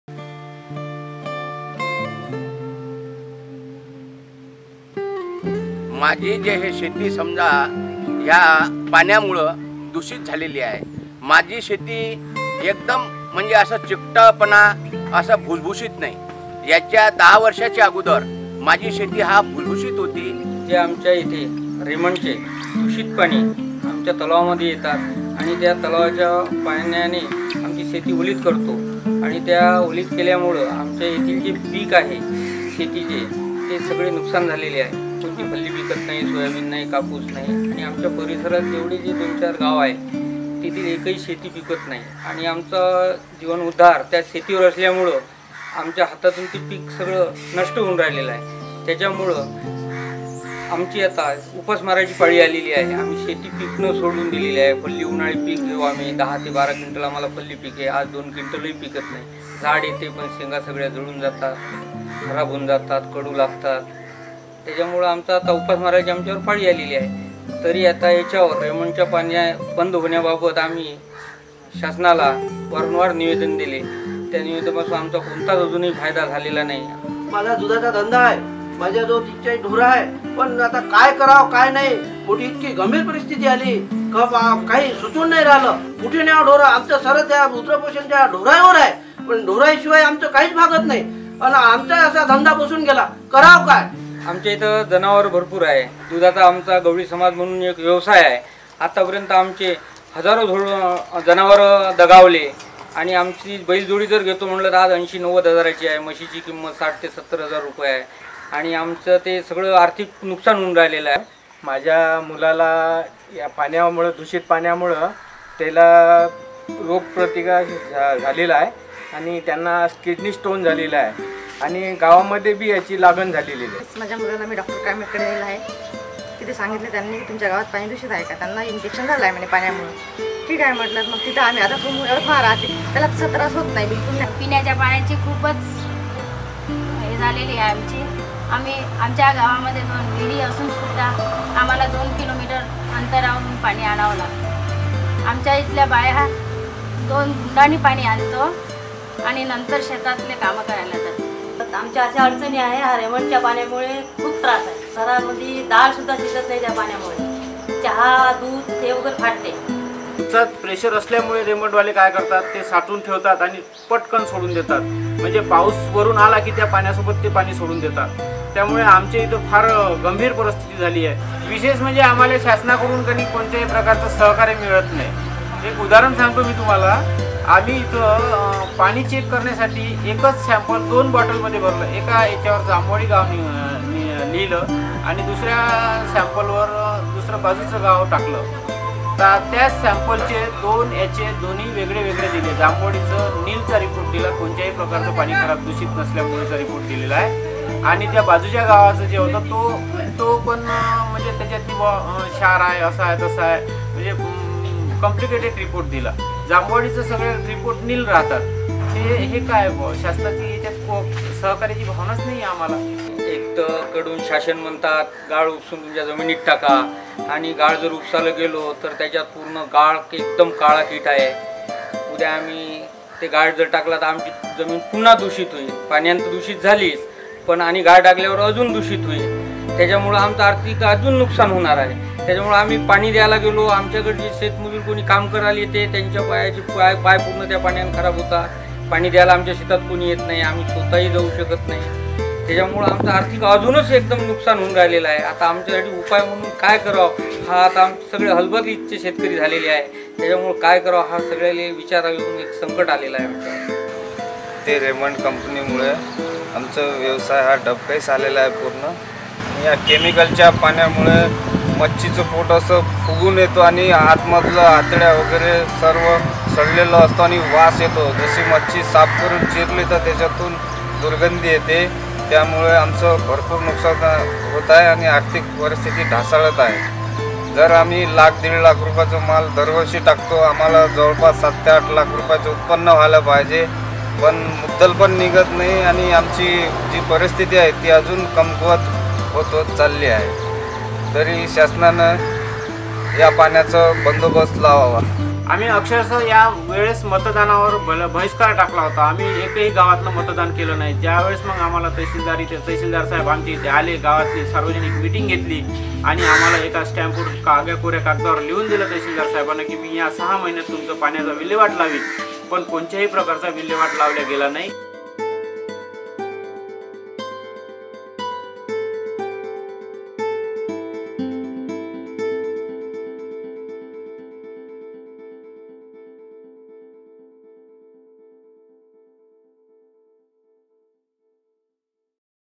The Poisonous Encounters-Impact of Raymond factory pollution in Yavatmal
This video is about the critical situation of some of the villages in Yavatmal affected by Raymond factory pollution.